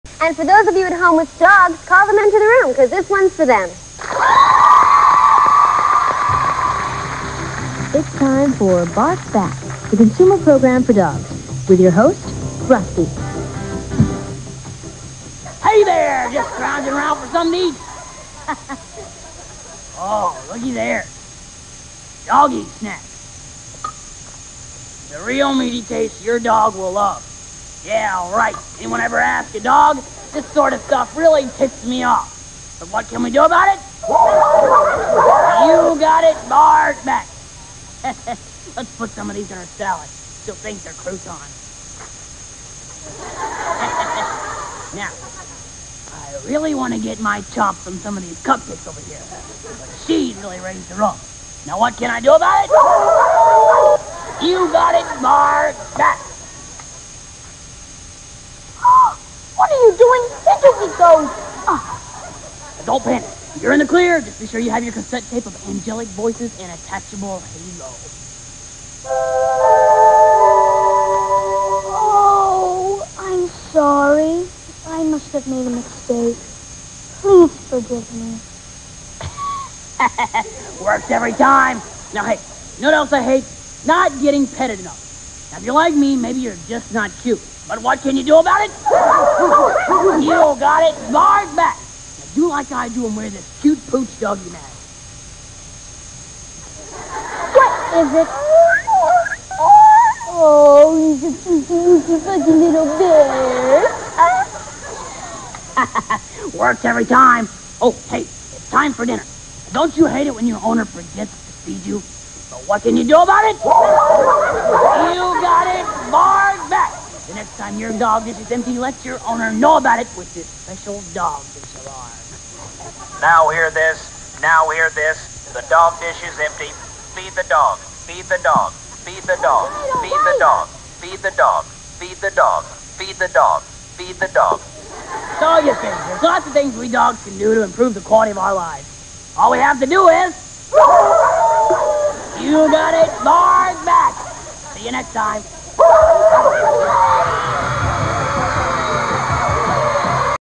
MMCS2 Bark Back
The show for dogs! Starring Chase & Jennifer. Introduced by Dee Dee, Kevin, and Mowava.